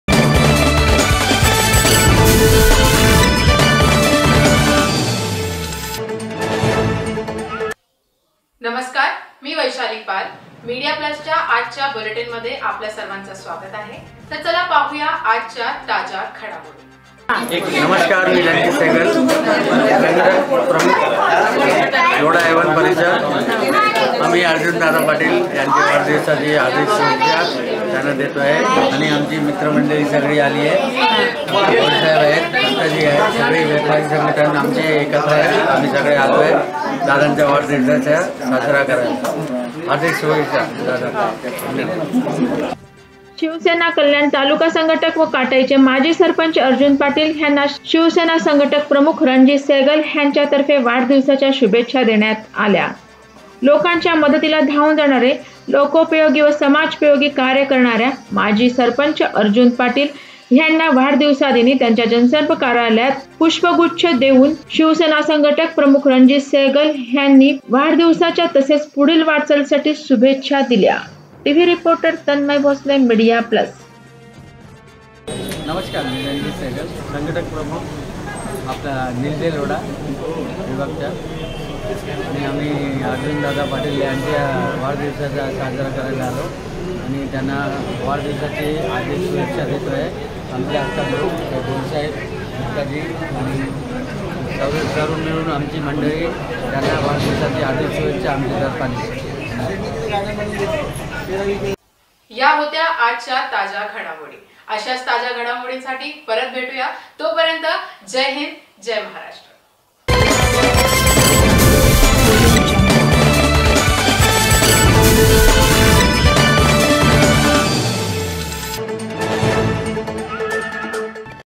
शिवसेना शाखा संघटक प्रमुख रणजित सहगल यांच्यातर्फेमा. सरपंच अर्जुन पाटील यांना वाढदिवसाच्या शुभेच्छा
शिवसेना शाखा संघटक प्रमुख रणजित सहगल यांच्यातर्फे शिवसेना कल्याण तालुका संघटक व माजी सरपंच अर्जुन पाटील यांना वाढदिवसाच्या शुभेच्छा |media plus